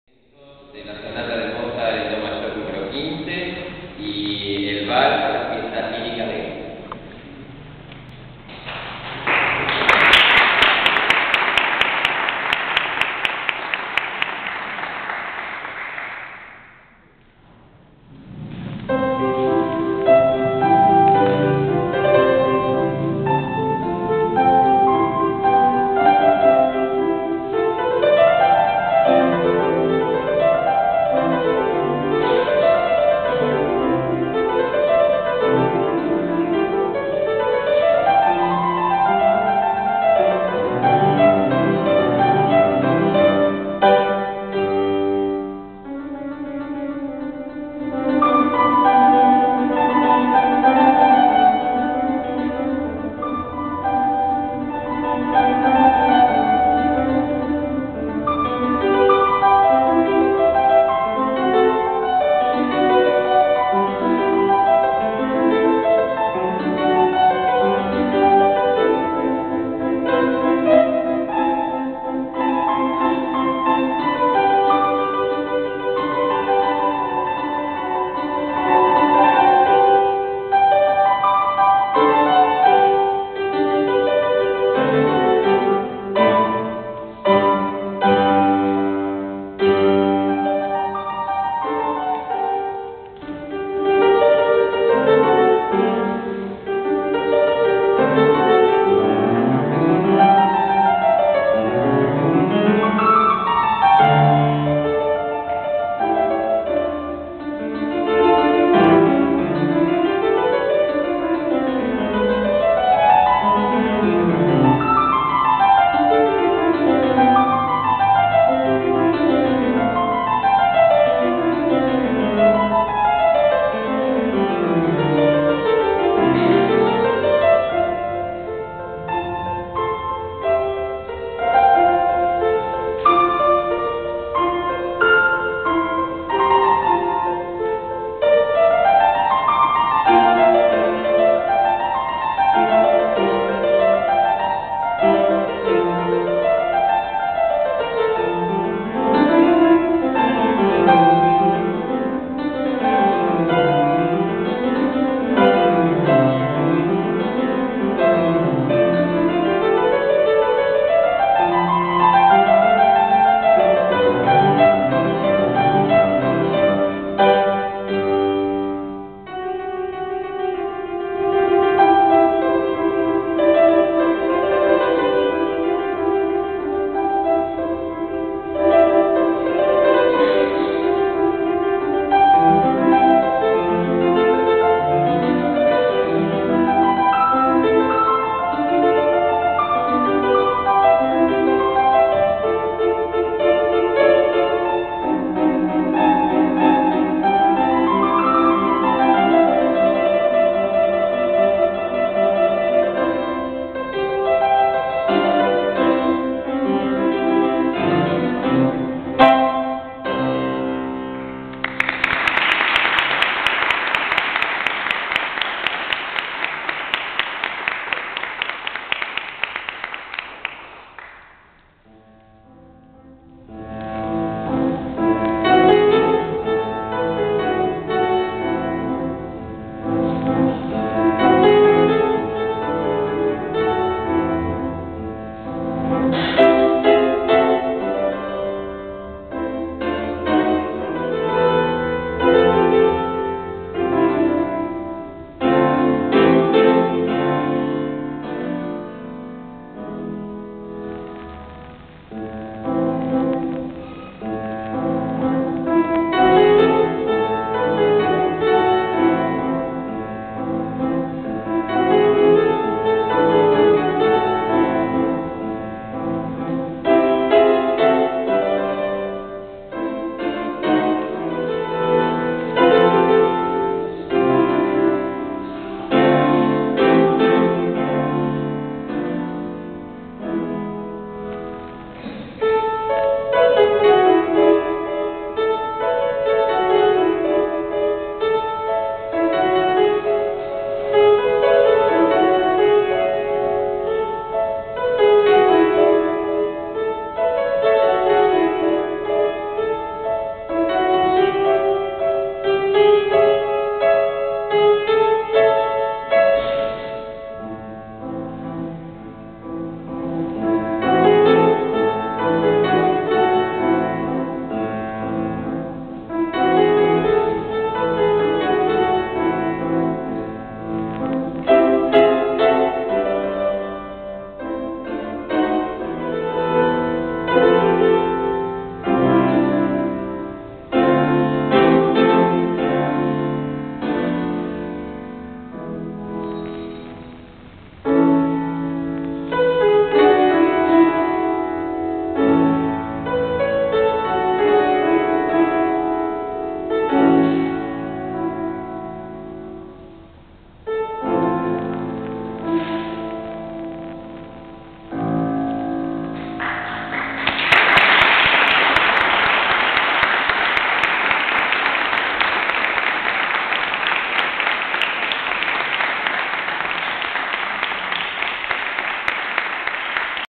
Concierto piano Agustín Seisdedos
En la escuela de Bellas Artes Carlos Morel de Quilmes el 15 de octubre de 2008
a cargo de Agustin Seisdedos